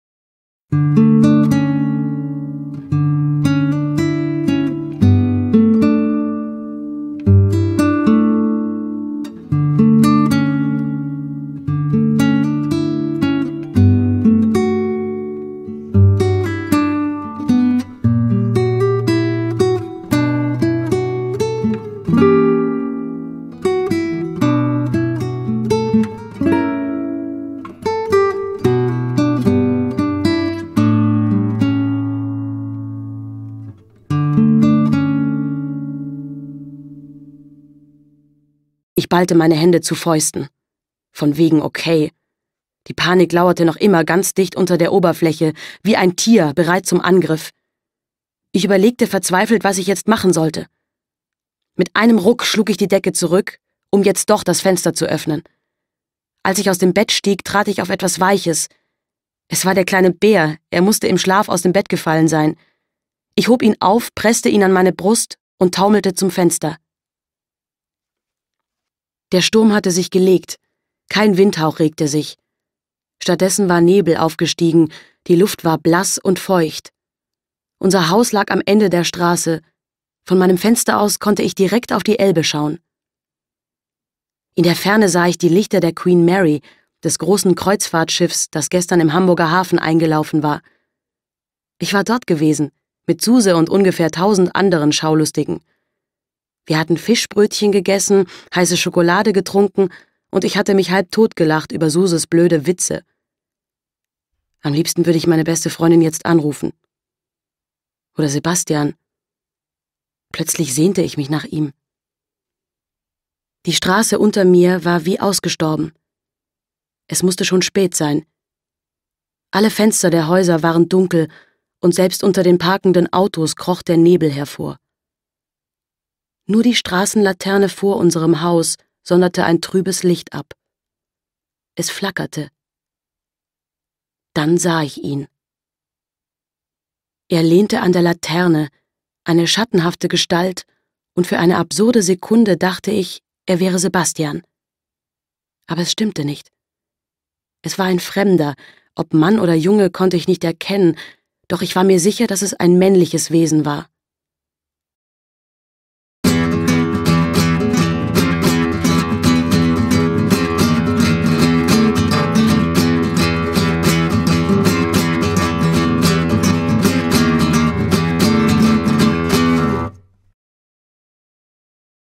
Isabel Abedi: Lucian - Hörbuch
Immer wieder taucht er in Rebeccas Umgebung auf: der geheimnisvolle Junge Lucian, der keine Vergangenheit hat und keine Erinnerungen. Sein einziger Halt ist Rebecca, von der er jede Nacht träumt. Und auch Rebecca spürt vom ersten Moment an eine Anziehung, die sie sich nicht erklären kann. So verzweifelt die beiden es auch versuchen, sie kommen nicht voneinander los. Aber bevor sie ihr gemeinsames Geheimnis erfahren können, werden sie getrennt, mit Folgen, die für beide grausam sind. Denn das, was sie verbindet, ist weit mehr als Liebe.

Das Hörbuch "Lucian" von Isabel Abedi, gesprochen von Julia Nachtmann und mit Musik von Eduardo Macedo, ist bei JUMBO/Goya libre erschienen. Mehr Informationen